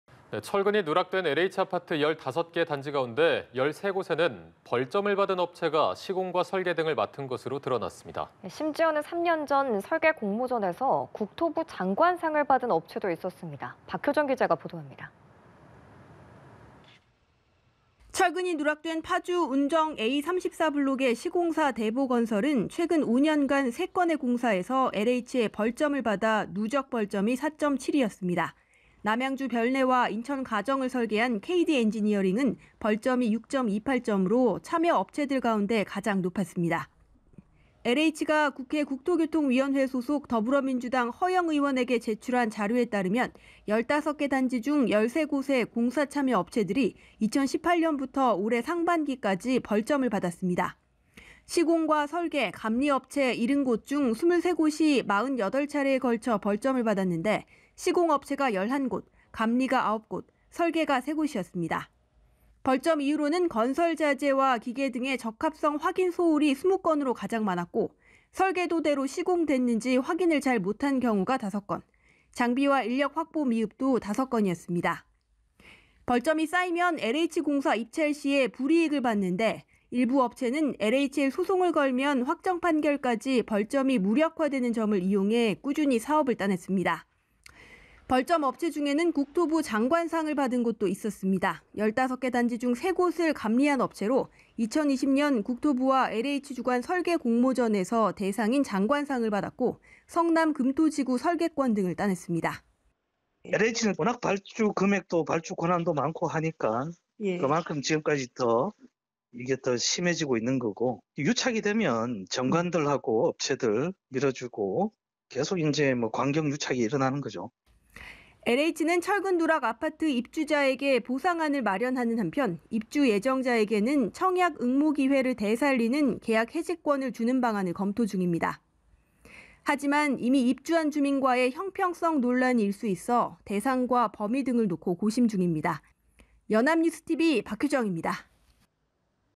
부실 시공 관련사엔 벌점업체 수두룩…장관상 업체도 / 연합뉴스TV (YonhapnewsTV)
부실 시공 관련사엔 벌점업체 수두룩…장관상 업체도
[뉴스리뷰]

[앵커]

철근이 누락된 LH 아파트 15개 단지 가운데 13곳에는 벌점을 받은 업체가 시공과 설계 등을 맡은 것으로 드러났습니다.

심지어는 3년 전 설계공모전에서 국토부 장관상을 받은 업체도 있었습니다.

박효정 기자가 보도합니다.

[기자]

철근이 누락된 파주 운정 A34 블록의 시공사 대보건설은 최근 5년간 3건의 공사에서 LH의 벌점을 받아 누적 벌점이 4.72였습니다.

남양주 별내와 인천 가정을 설계한 케이디엔지니어링은 벌점이 6.28 점으로 참여 업체들 가운데 가장 높았습니다.

LH가 국회 국토교통위원회 소속 더불어민주당 허영 의원에게 제출한 자료에 따르면 15개 단지 중 13곳의 공사 참여 업체들이 2018년부터 올해 상반기까지 벌점을 받았습니다.

시공, 설계, 감리업체 70곳 중 23곳이 48차례에 걸쳐 벌점을 받았는데, 시공업체가 11곳, 감리가 9곳, 설계가 3곳이었습니다.

벌점 이유로는 건설 자재·기계 등의 적합성 확인 소홀이 20건으로 가장 많았고, 설계도대로 시공됐는지 확인을 잘 못한 경우가 5건, 장비·인력 확보 미흡도 5건이었습니다.

벌점이 쌓이면 LH 공사 입찰 시 불이익을 받는데, 일부 업체는 LH에 소송을 걸면 확정판결까지 벌점이 무력화되는 점을 이용해 꾸준히 사업을 따냈습니다.

벌점 업체중에는 국토부 장관상을 받은 곳도 있었습니다.

15개 단지 중 3곳을 감리한 업체로, 2020년 국토부와 LH 주관 설계 공모전에서 대상인 장관상을 받았고, 성남금토지구 설계권 등을 따냈습니다.

[권오인 / 경제정의실천시민연합 정책실장] "LH는 워낙 발주금액도 권한도 많으니까 그만큼 심한 것이고, 유착이 되면 전관들이 업체들 밀어주고 계속 관경 유착이 일어나는거죠."

LH는 철근 누락 아파트 입주자 대상 보상안을 마련하는 한편, 입주 예정자에게는 청약 응모 기회를 되살리는 계약해지권의 부여를 검토 중입니다.

하지만 이미 입주한 주민과 형평성 논란이 일 수 있어 대상과 범위 등을 놓고 고심 중입니다.